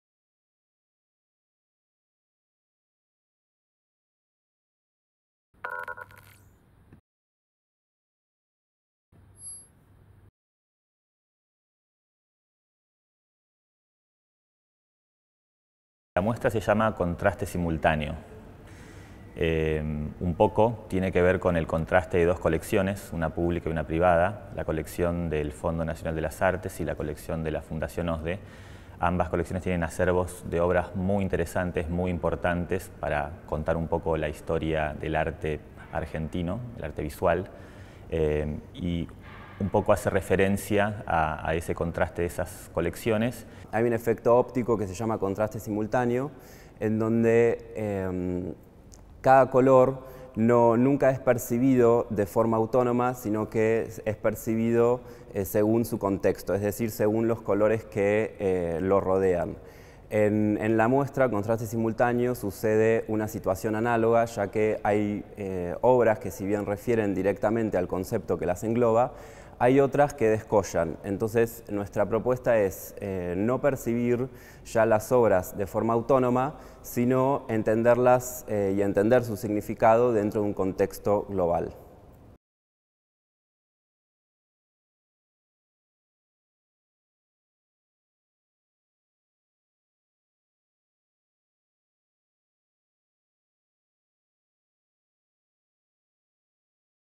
2018. Contraste Simultáneo.
En el año 2018 se exhibía, en las salas de Suipacha del Espacio de Arte, la exposición Contraste Simultáneo, una selección de 105 obras que ponía en diálogo las colecciones del Fondo Nacional de las Artes y la Fundación OSDE. Sus curadores, Juan Balza y Gonzalo Maciel, dividieron la exposición en seis núcleos conceptuales. Mientras en aquella exposición del 2018 las obras de Mariano Sapia y Nicolás Martella se agrupaban dentro del núcleo "La fábrica", las de Beatriz Moreiro y Adriana Bustos como parte de "Documentación de lo real" y la pieza de Juan Andrés Videla en el apartado de "Artificios"; hoy, algunas de esas obras junto a otras conforman Paisaje, la primera exposición del ciclo A través del cristal. Esta vez estas imágenes nos llevan a reflexionar, en su conjunto, sobre los diferentes paisajes que representan, "...nos hablan de un tiempo histórico y sus problemáticas; de coincidencias y contradicciones atravesadas por nuestro tiempo en el campo y la ciudad. Reflexionan sobre el trabajo rural, la crisis económica, la agresión a la naturaleza, el ritmo acelerado de la urbe." en palabras de la curadora Micaela Bianco.